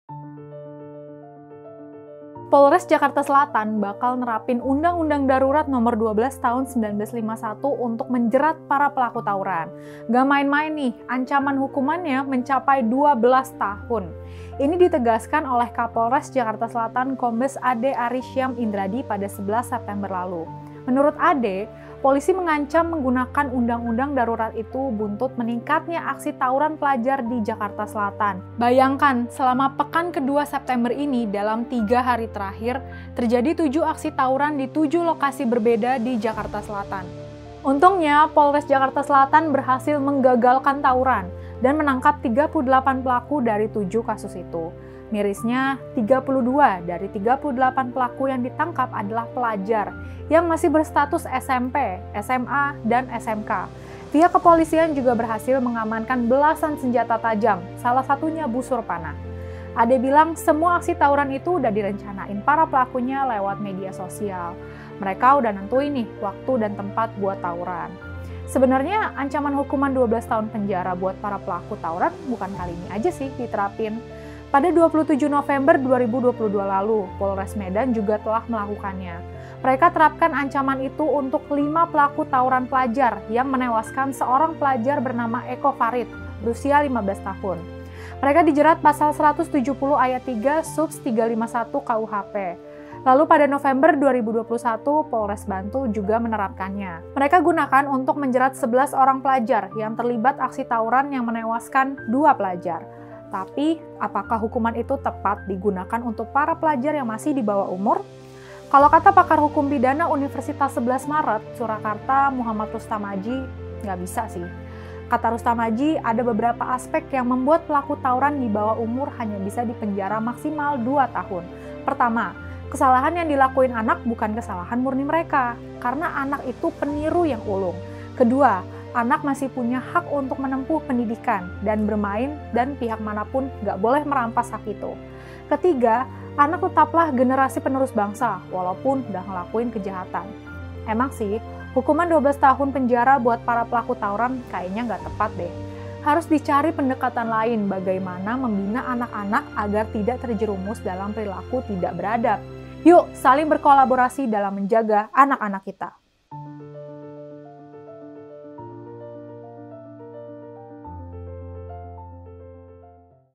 Apakah Tepat Pelaku Tawuran Diancam 12 Tahun Penjara? | Cindy Florencine
Apakah Tepat Pelaku Tawuran Diancam 12 Tahun Penjara?

Polres Jakarta Selatan rencananya bakal netapin UU Darurat No 12 tahun 1951 dalam menjerat para pelaku tawuran nih. Dengan UU itu maka pelaku bisa terancam hukuman maksimal 12 tahun penjara. Hukuman ini dirasa beberapa pihak kurang efektif karena akar permasalahannya bukan pada anak, melainkan berbagai pihak terkait dalam membina anak-anak agar tak terjerumus perbuatan tak beradab. Yuk saling berkolaborasi menjaga anak-anak kita!